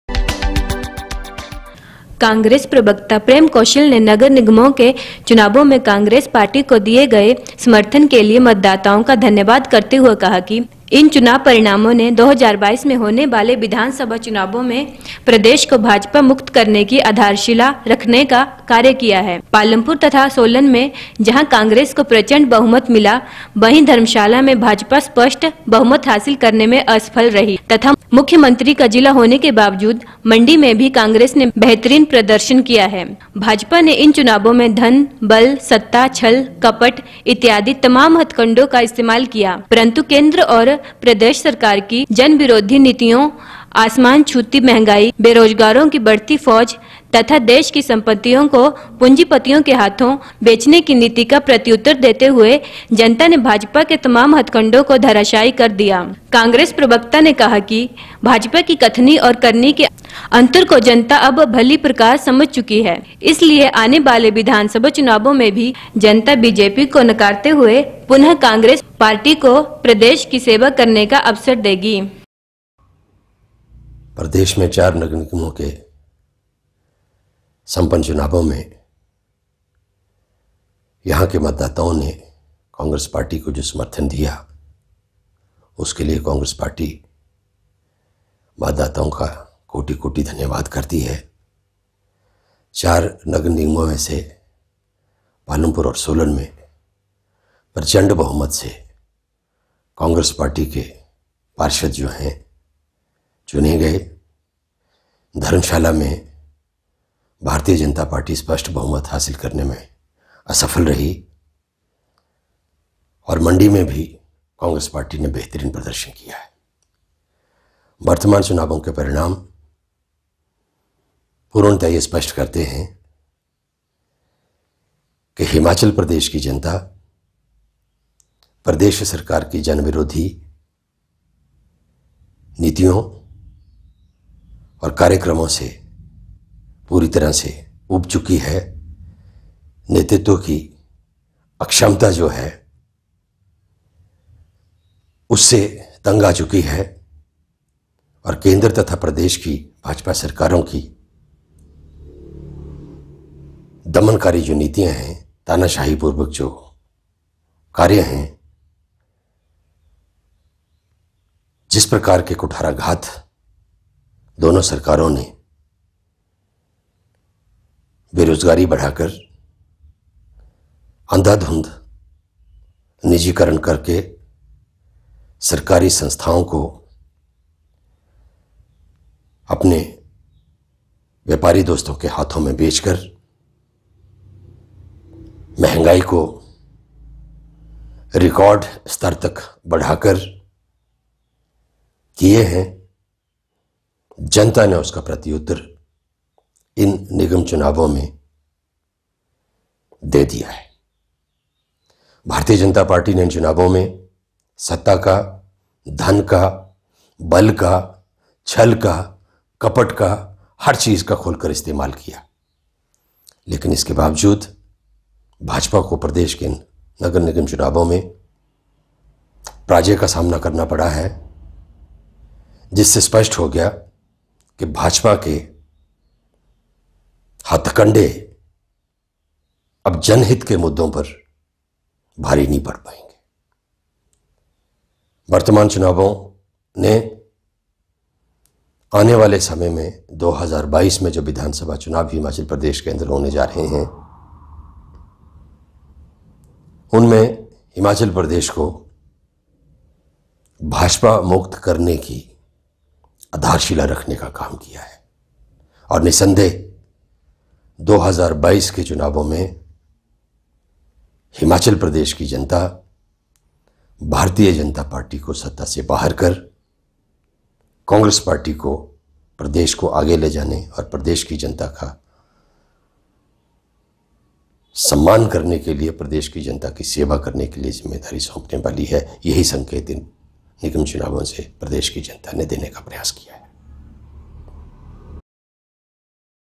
प्रेम कौशल ने नगर निगम चुनाबों में कांग्रेस पार्टी को दिए गए समर्थन के लिए मतदाताओं का धन्यवाद किया
कांग्रेस प्रवक्ता प्रेम कौशल ने नगर निगमों के चुनाबों में कांग्रेस पार्टी को दिए गए समर्थन के लिए मतदाताओं का धन्यवाद करते हुए कहा कि इन चुनाब  परिणामों ने 2022 में होने बाले विधानसभा चुनावों में प्रदेश को भाजपा मुक्त करने की आधारशिला रखने का कार्य किया है